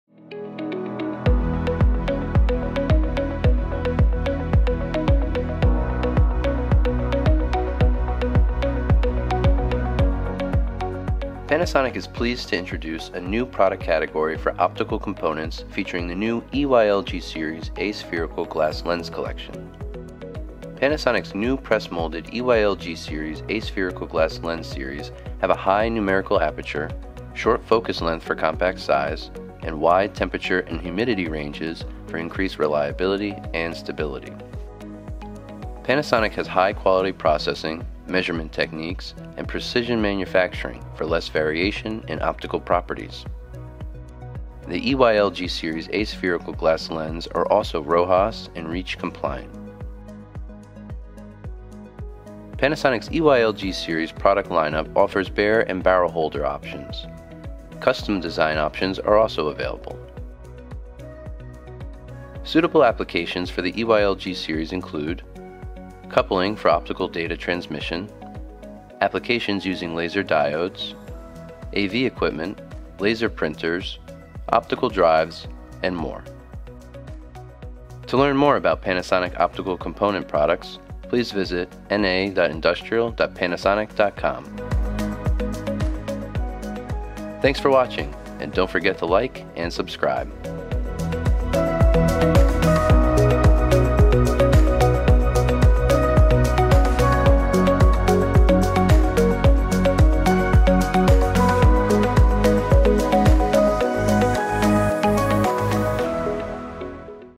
Quick Clips: Aspherical Glass Lenses
Panasonic's EYL-G Series Aspherical Glass Lenses have a high numerical aperture, short focal length for compact size and wide temperature and humidity ranges for increased reliability stability. Panasonic has high-quality processing, measurement techniques and precision manufacturing for less variation in optical properties. Bare and barrel holder options are available as well as custom design capability.